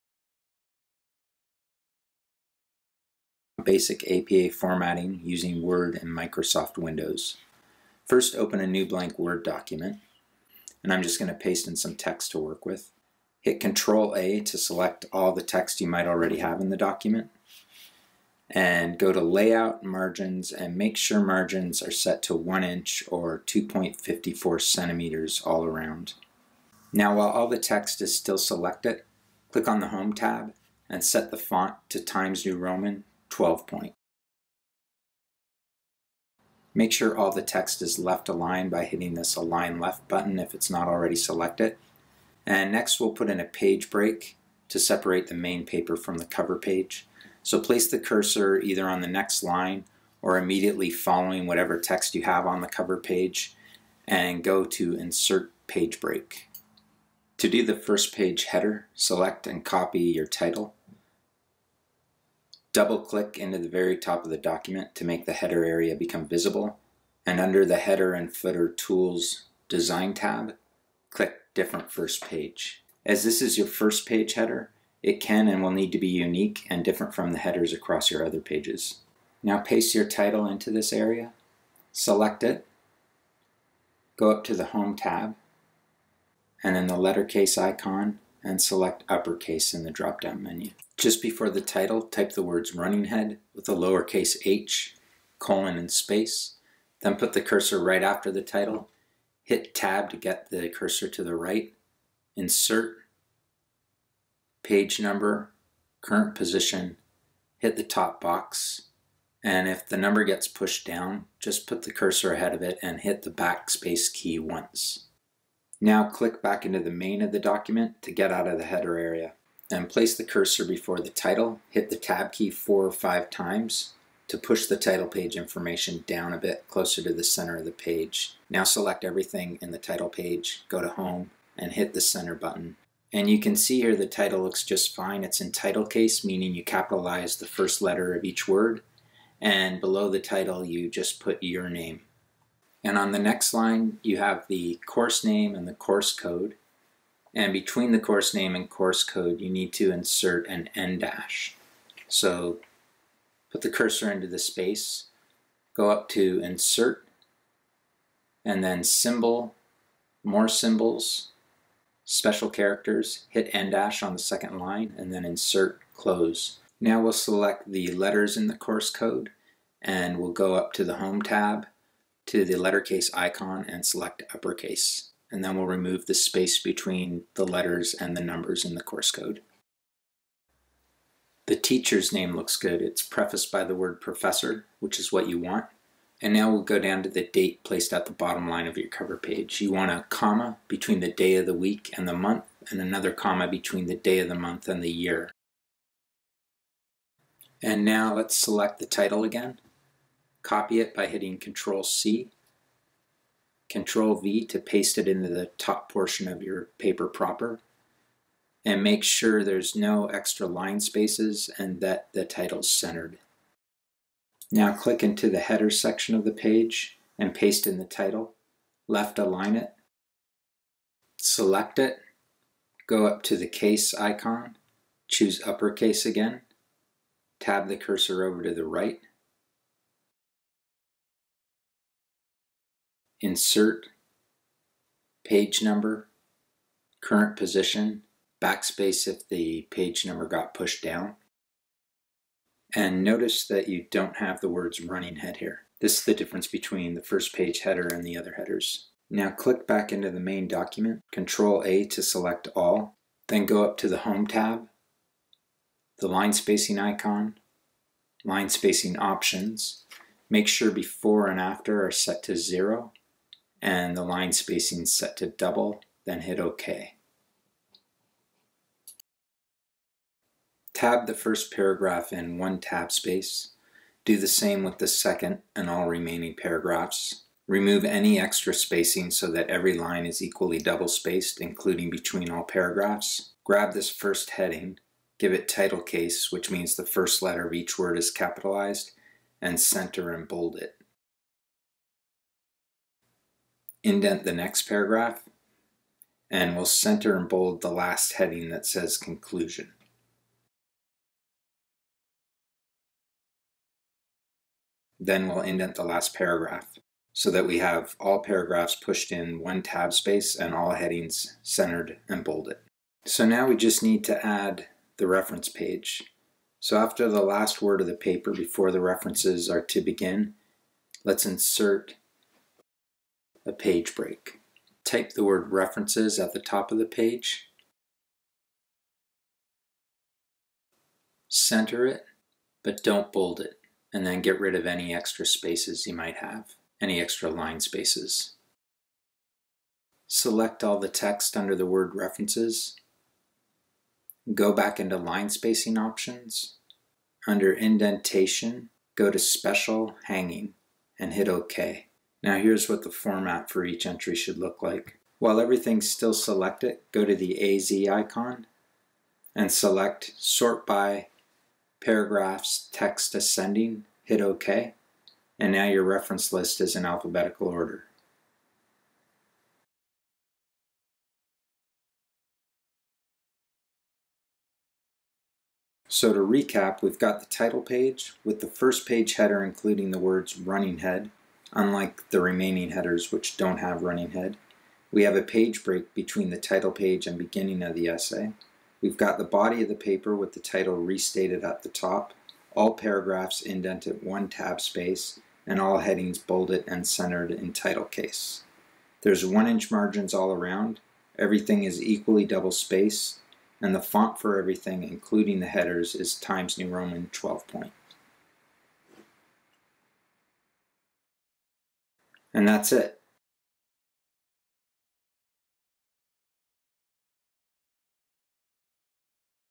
How to Format an APA Paper in Word 2019 on PC
Describes setting up an APA paper in Microsoft Word 2019